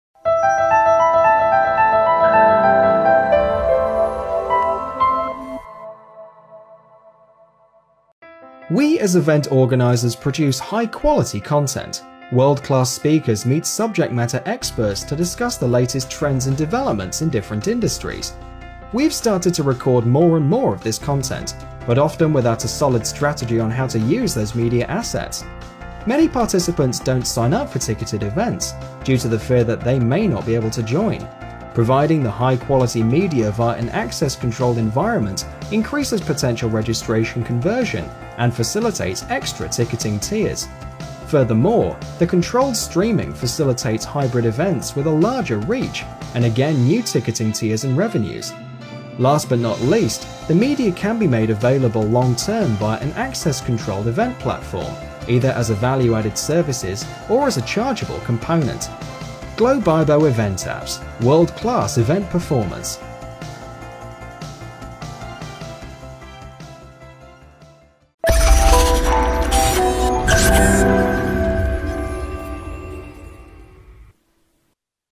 Event Media Management & Ticketing Options by Globibo
Case Study : Leveraging video content created during an event for creating new ticketing tiers or participation models.